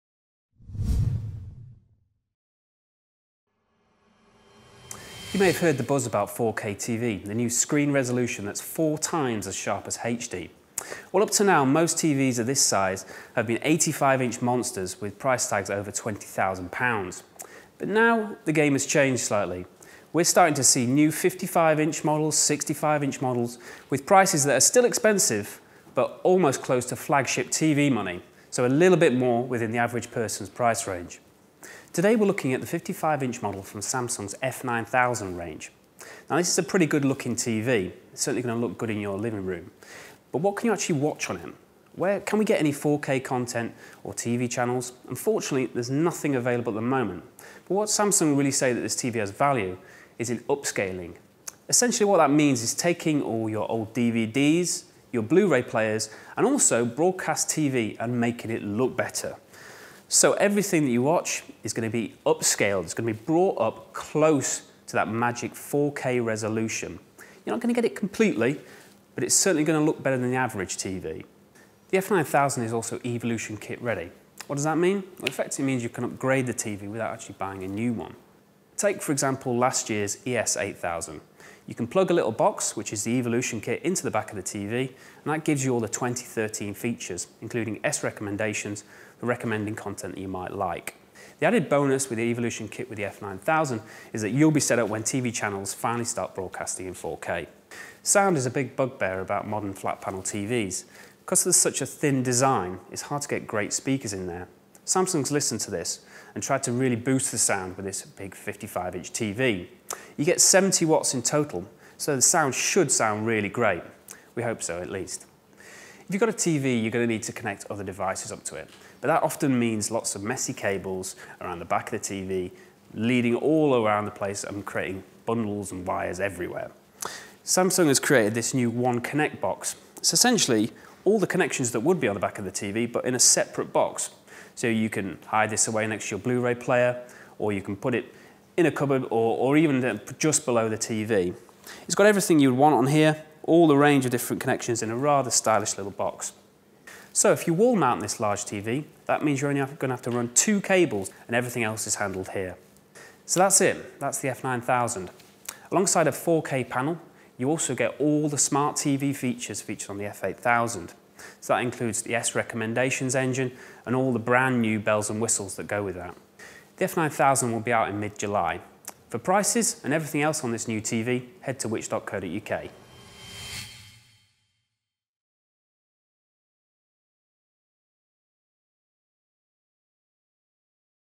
Samsung F9000 4K TV - Which? first look
We take a look at the new Samsung F9000 55-inch TV, a 4K TV set that is close to affordable.

Ultra HD, also known as 4K, is a TV screen resolution of over eight million pixels (3840 x 2160 pixels), around four times that of Full HD (1920 x 1080 pixels).

The first 4K TVs were unleashed last year, but at over 80-inches in screen size and price tags north of £20,000, they were only for the super-rich. Now Samsung has launched a 55-inch Samsung F9000 series 4K TV for £3,999.99; an almost affordable price. We took a first look at the new TV.